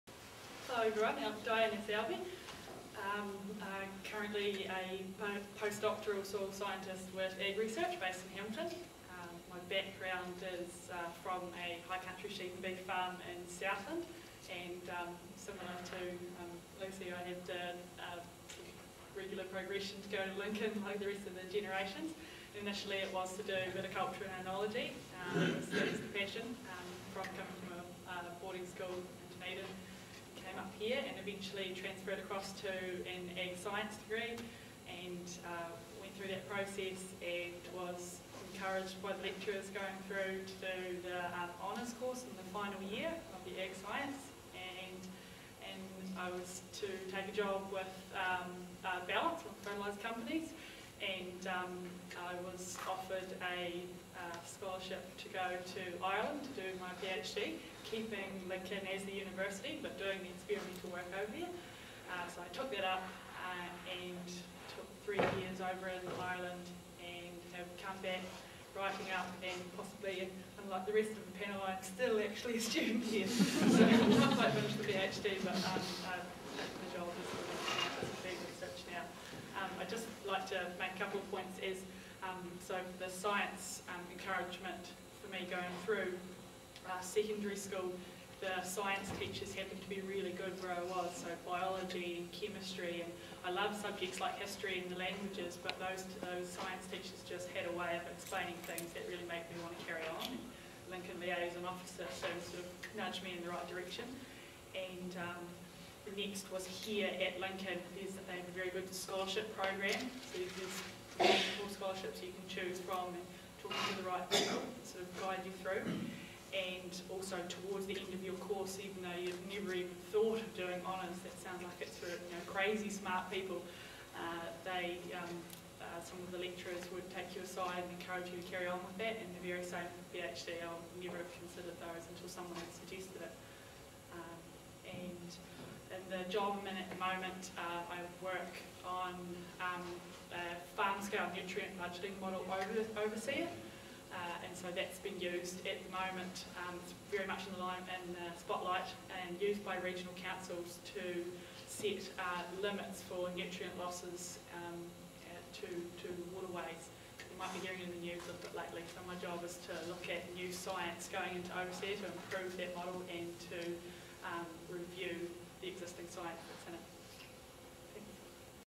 Diana Selbie - Post-doctoral Scientist, AgResearch
Diana Selbie studied for a Bachelor of Agricultural Science (Hons) at Lincoln University and won a scholarship to pursue a PhD in Ireland. This led Diana to her current role as post-doctoral scientist at AgResearch.